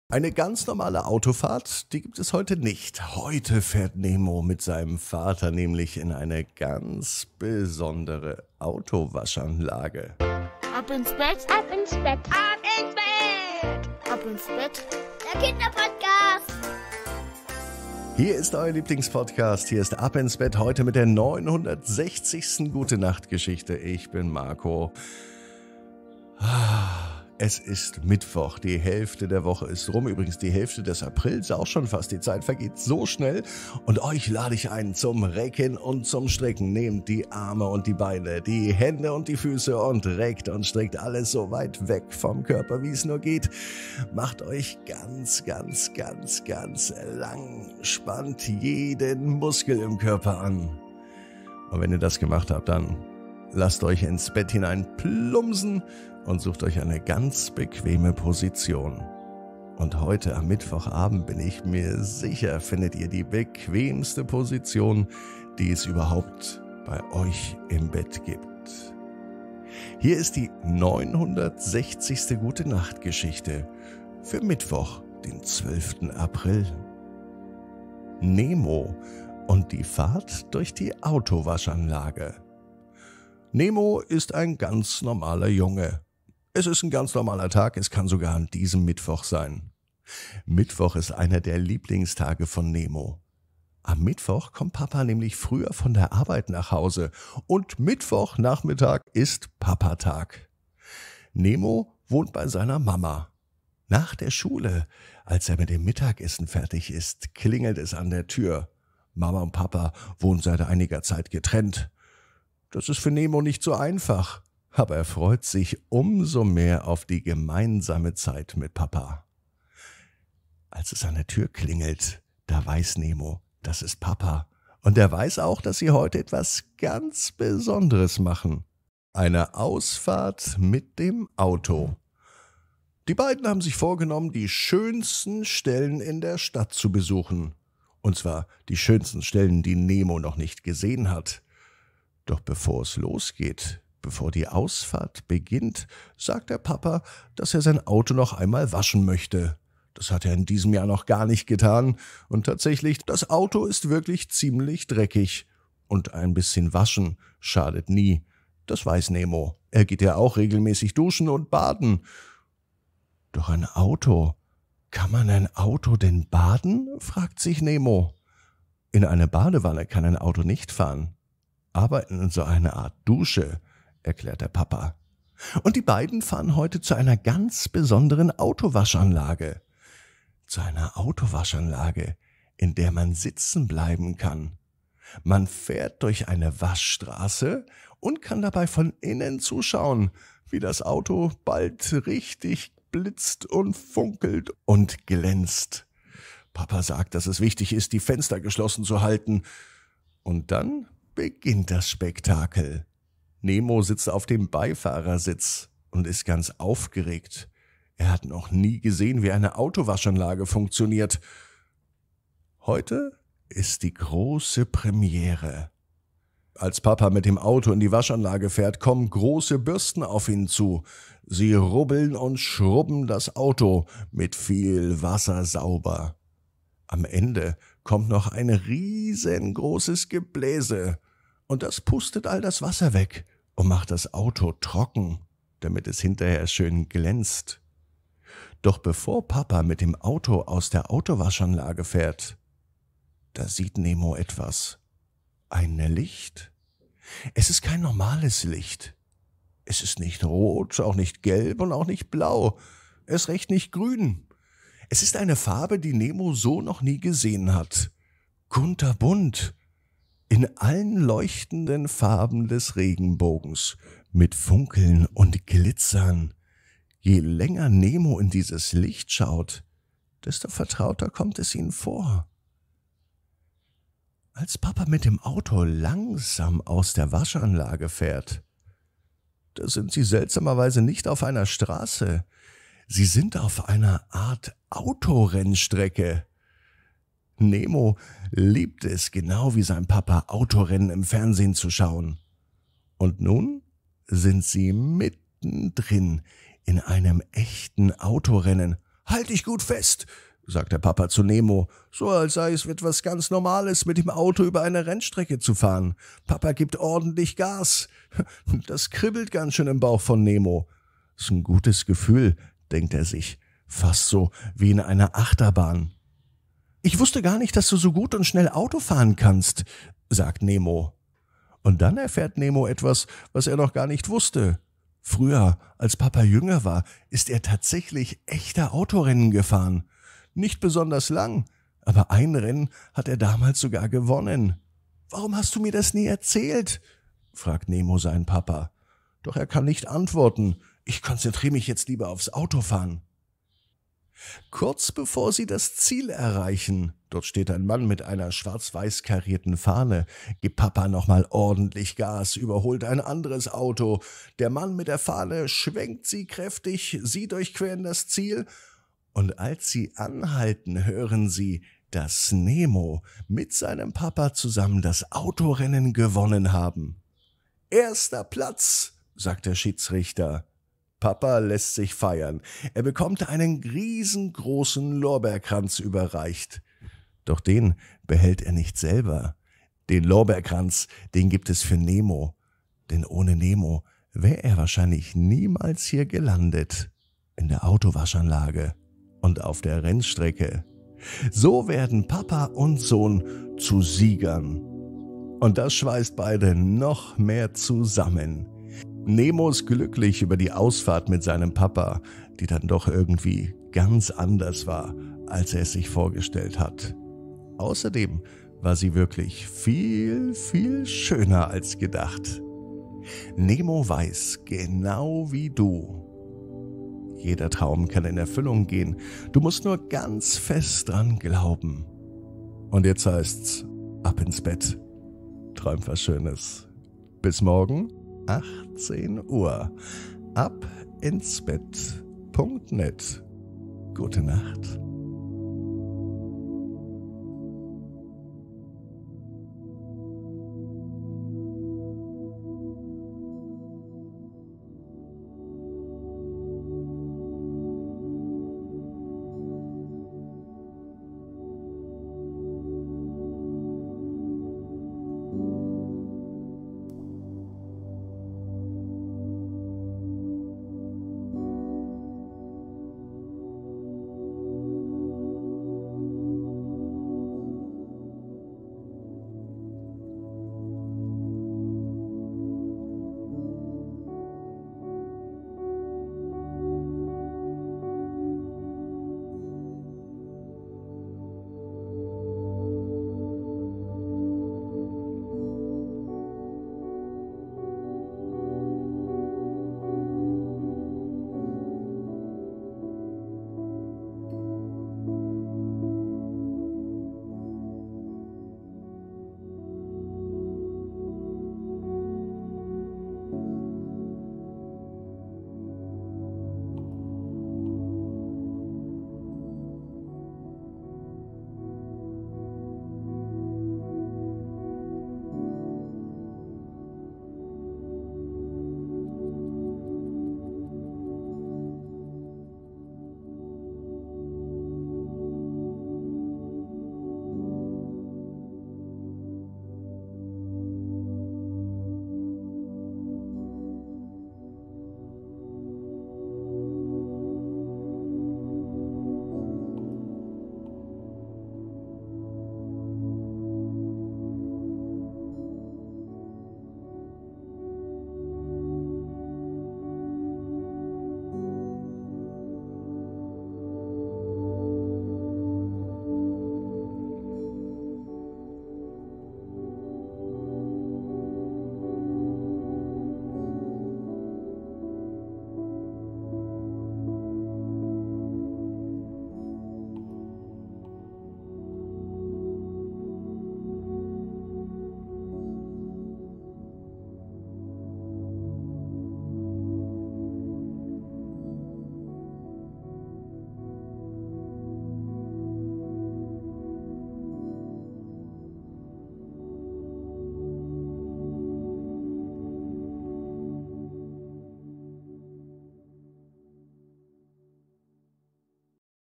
#960 Nemo und die Fahrt durch die Autowaschanlage - Ab ins Bett - Die tägliche Gute Nacht Geschichte
Nemo verbringt den Mittwoch mit Papa. Heute machen sie einen Ausflug mit dem Auto. Der Zwischenstopp in der Autowaschanlage bringt einige Überraschungen mit sich. Die ganze Gute Nacht Geschichte hört ihr in der neuen Gute Nacht Geschichte bei Ab ins Bett mit Marco König.